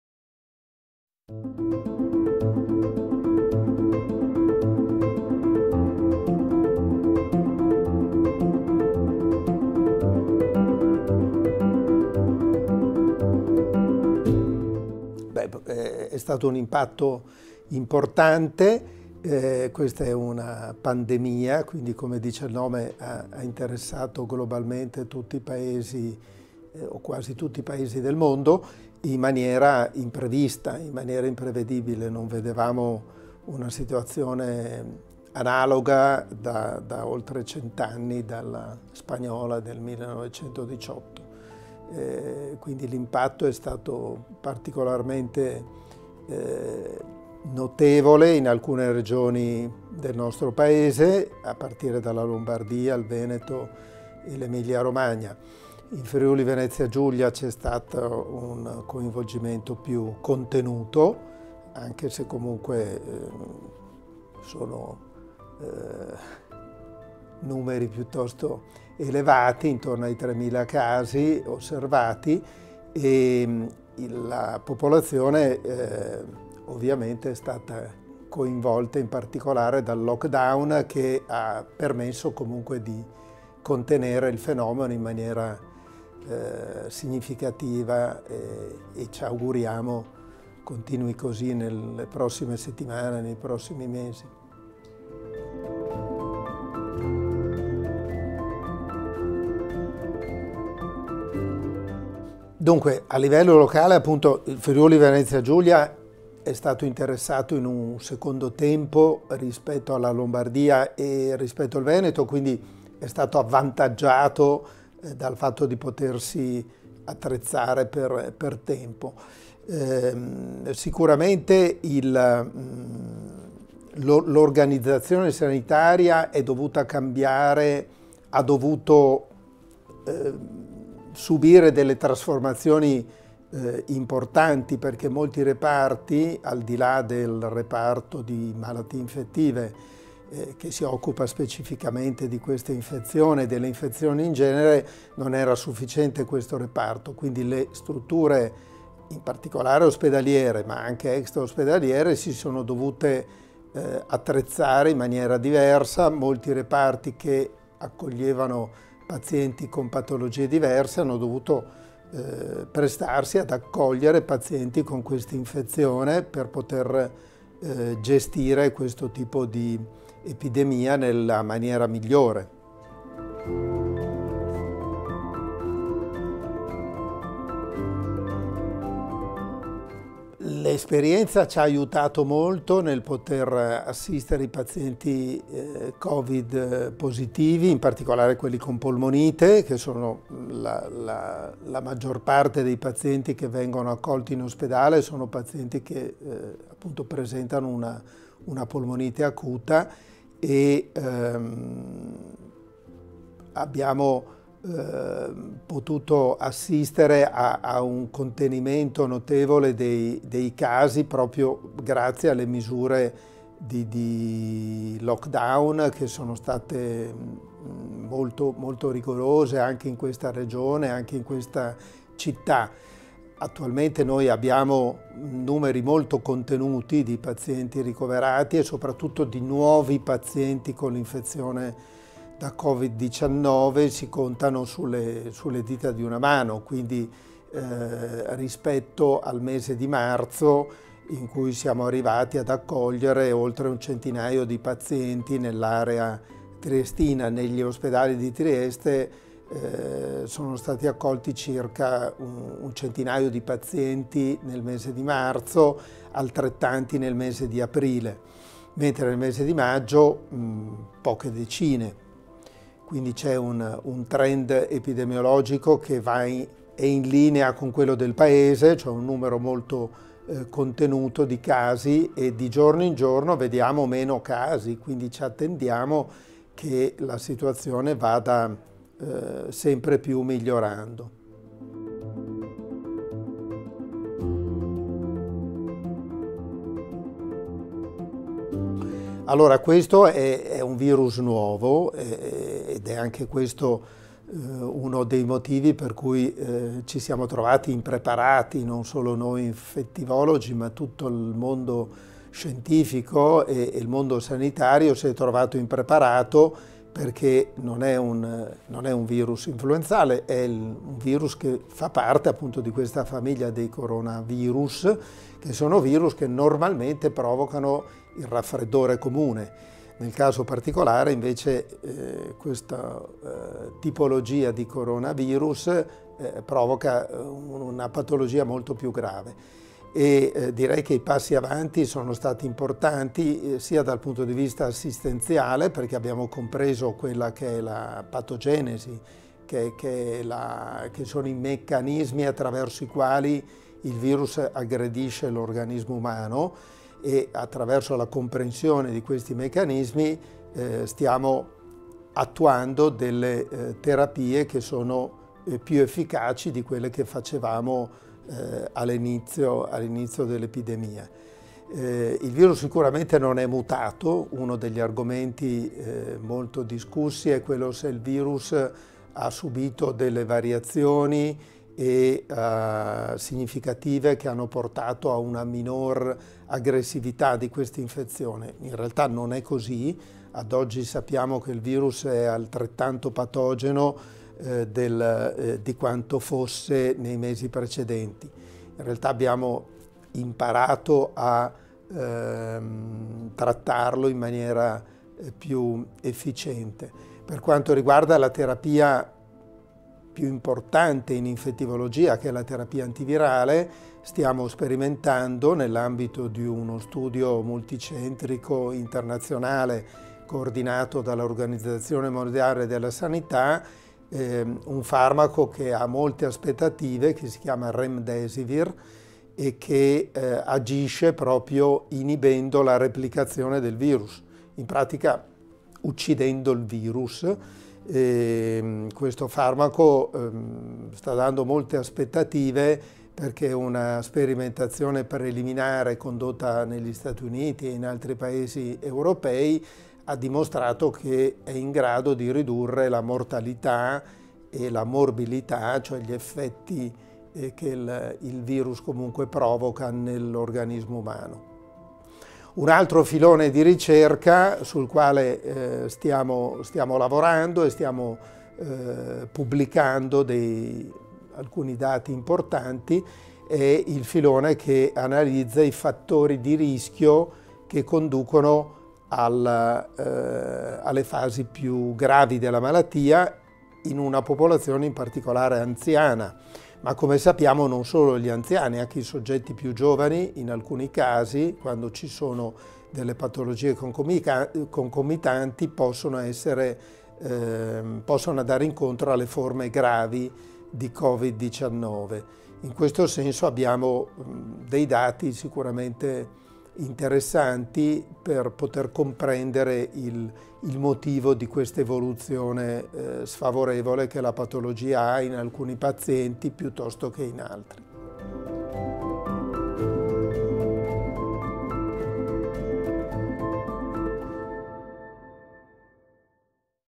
Impatto della pandemia Covid19 in Friuli Venezia Giulia e a Trieste (attività clinico-assistenziale)
docente: prof. Luzzati